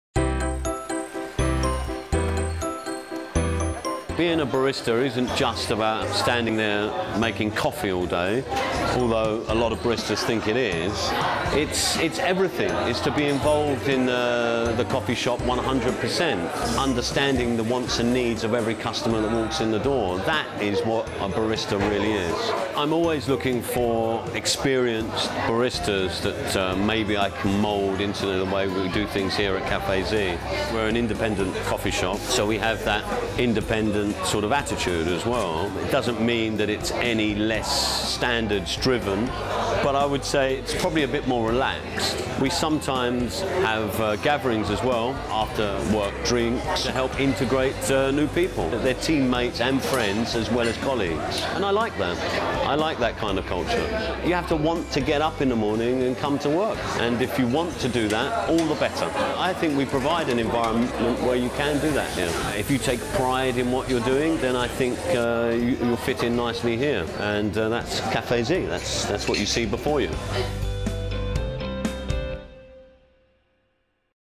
CAFE ZEE - Star Company @ JOB TODAY
CAFE ZEE is a great speciality coffee shop and micro-roastery. With 2 locations in London, they have a family-like team of 20 members who think it truly is a great place to work. They are always on the lookout for experienced baristas who want to express themselves and flourish within a relaxed work environment.

Apply to CAFE ZEE's vacancies on the JOB TODAY app!